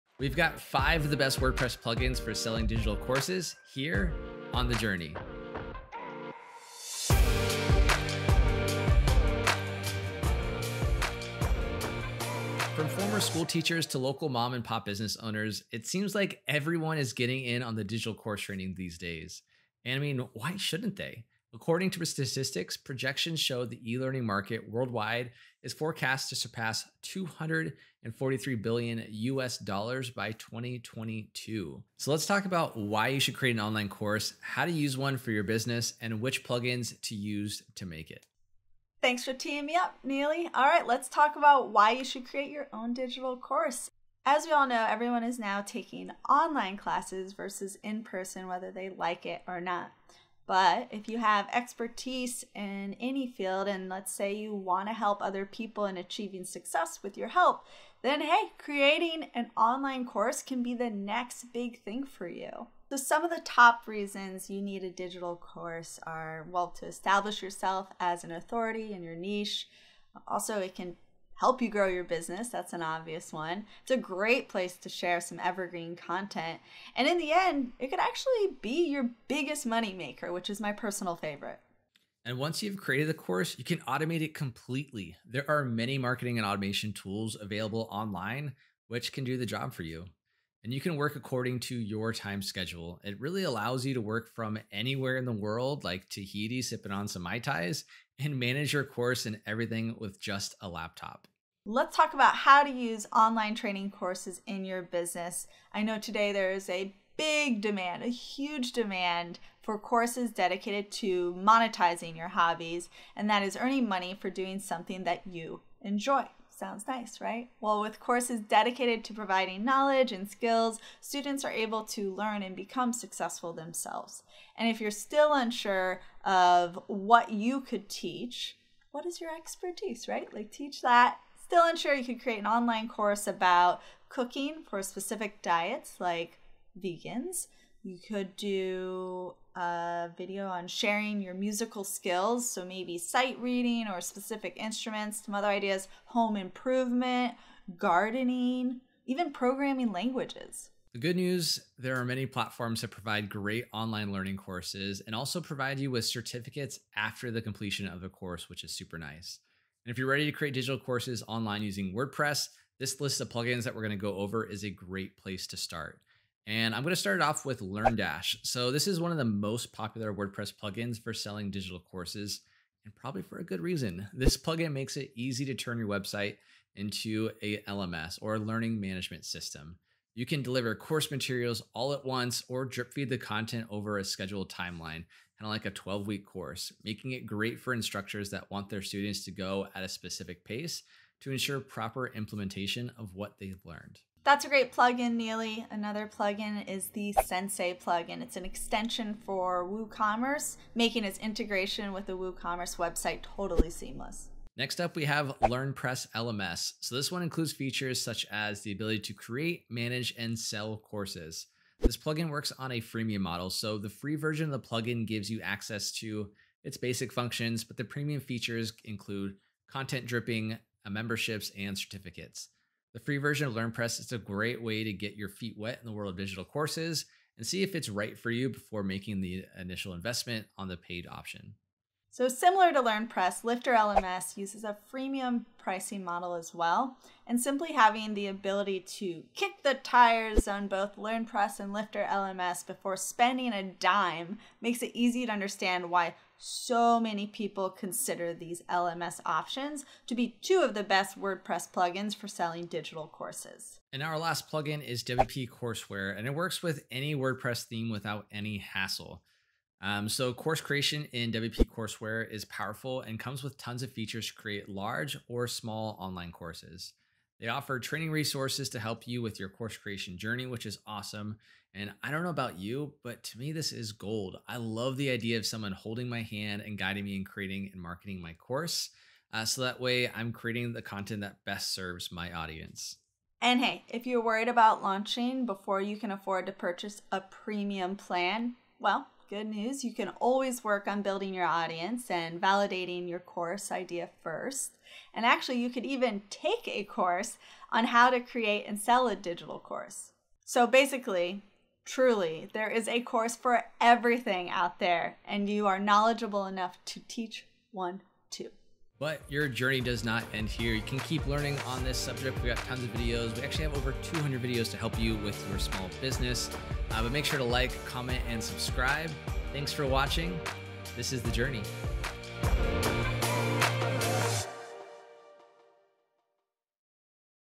5 Best WordPress Plugins for Selling Digital Courses | The Journey
With over 455 million websites using WordPress, many educators are looking to leverage their existing WordPress sites for digital learning. Luckily, there are quite a few helpful WordPress plugins for selling digital courses to help those educators out.

0:14 5 best WordPress plugins for selling digital courses
1:05 Why create a digital course
2:49 LearnDash
3:17 Sensei
3:29 LearnPress
3:54 LifterLMS
4:18 WP Courseware

Today there is a big demand for courses dedicated to monetizing your hobbies. That is, earning money from doing something you enjoy. With courses dedicated to providing knowledge and skills, students are able to learn and become successful themselves.

Best part, once you have created the course, you can automate it completely. There are many marketing and automation tools available online which can do the job for you. You can work according to your time schedule. It allows you to work from anywhere in the world and manage your course and everything with just a laptop.

If you’re ready to create digital courses online using WordPress, this list of plugins is a great place to start. So, without further ado, in no particular order, here are the top-recommended plugins we recommend:

LearnDash - More than just a plugin, LearnDash is trusted to power the learning programs for major universities, small to mid-size companies, startups, entrepreneurs, and bloggers worldwide.

Sensei - integrates effortlessly with your WordPress site, making it easy to create courses, lessons and quizzes. And with the WooCommerce Paid Courses extension, you can sell your courses using the most popular eCommerce platform on the web.

LearnPress - an excellent user interface to create a course with options you need. You can find it easy to make a full curriculum layout as well as edit and maintain it. You can also export course and import to other website which also using LearnPress.

LifterLMS - Make simple online courses, advanced multi-instructor training based membership websites, and everything in between. Whether you call it a course, membership site, or learning management system, Lifter LMS can help you make it.

WP Courseware - Customize the look and style of your course to match your website with a few clicks, no coding necessary. It’s also compatible with your video hosting solution of choice — YouTube, Wistia, Vimeo, or S3 Media Maestro.

There truly is a course for everything, in fact, you could always take a course on how to create and sell a digital course. We hope this post inspired you to plan a course launch of your own.